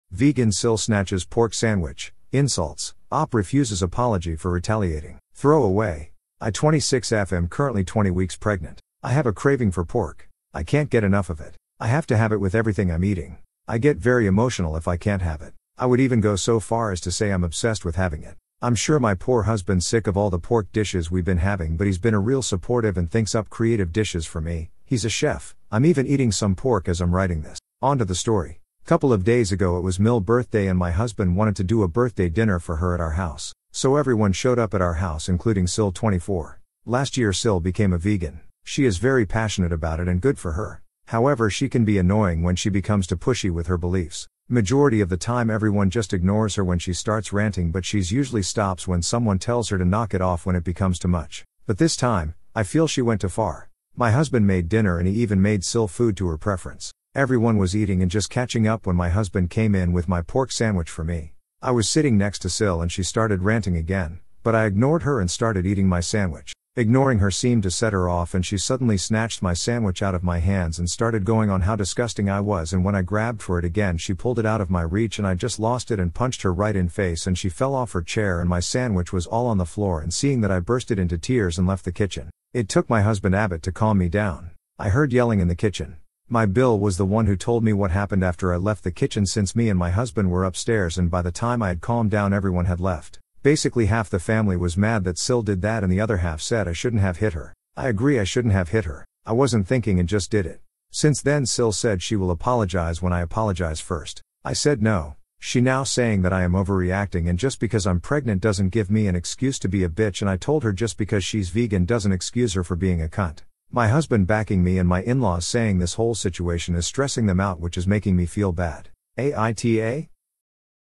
Vegan SIL snatches pork sandwich, insults; OP refuses apology for retaliating.
Pregnant woman loses temper when vegan SIL snatches her pork sandwich, leading to a family feud. She refuses to apologize for retaliating.